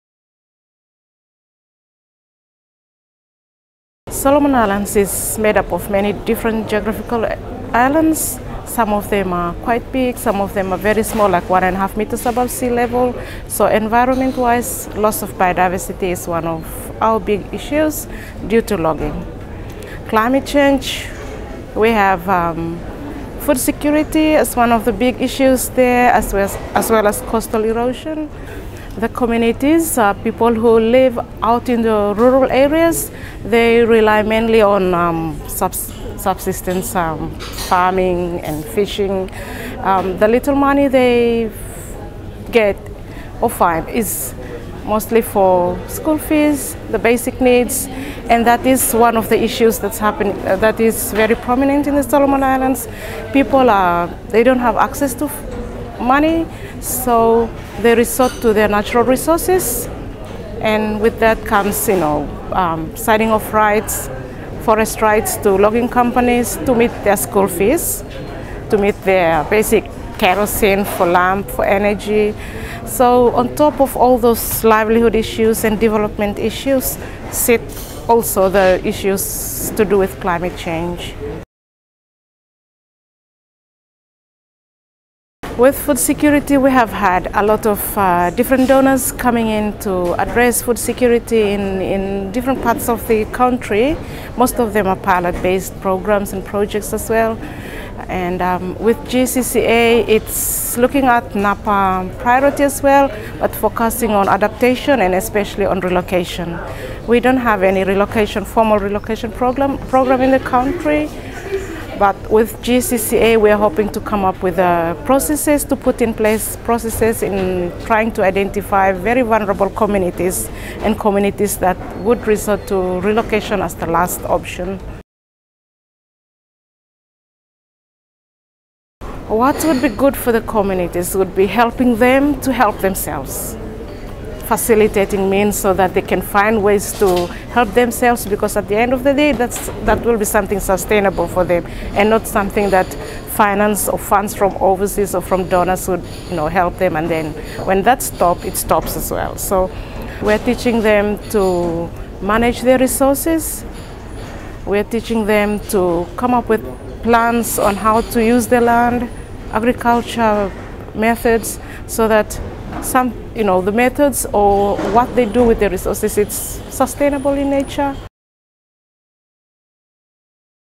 GCCA Global Learning Event - Nesta Leguvaka (September 2012)
Nesta Leguvaka works for the Solomon Islands Climate Adaptation Programme.

This interview was filmed at the first GCCA Global Learning Event in September 2012. For more information on this event, please visit the GGCA website, or read the Voices and Views about this event